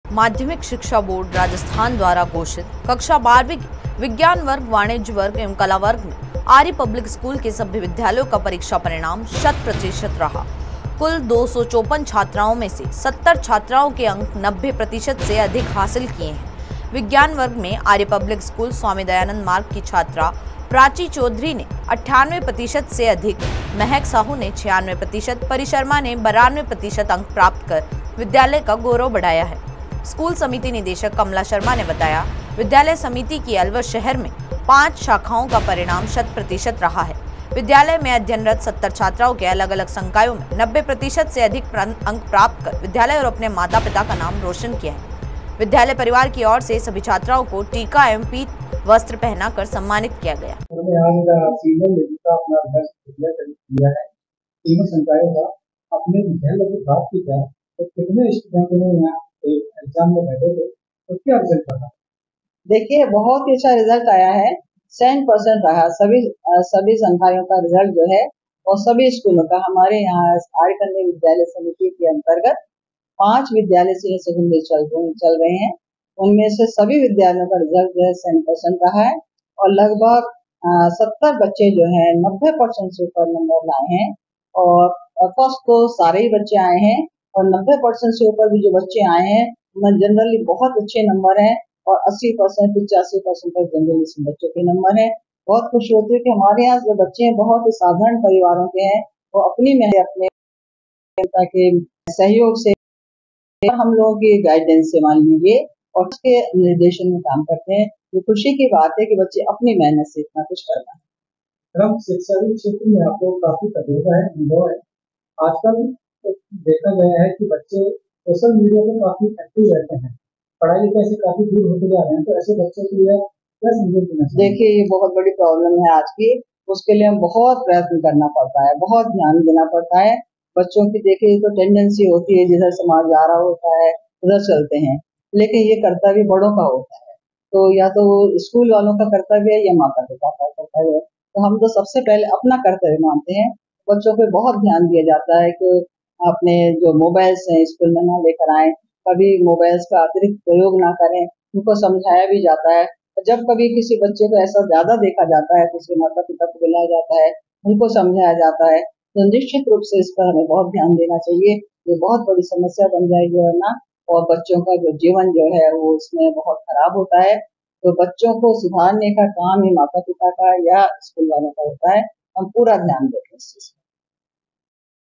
आर्य पब्लिक स्कूल की प्राची चौधरी ने 98.60 प्रतिशत अंक प्राप्त कर जिले में टॉपर रही
Popular TV के साथ देखिये देश-विदेश की सभी महत्वपूर्ण और बड़ी खबरें | Watch the latest Hindi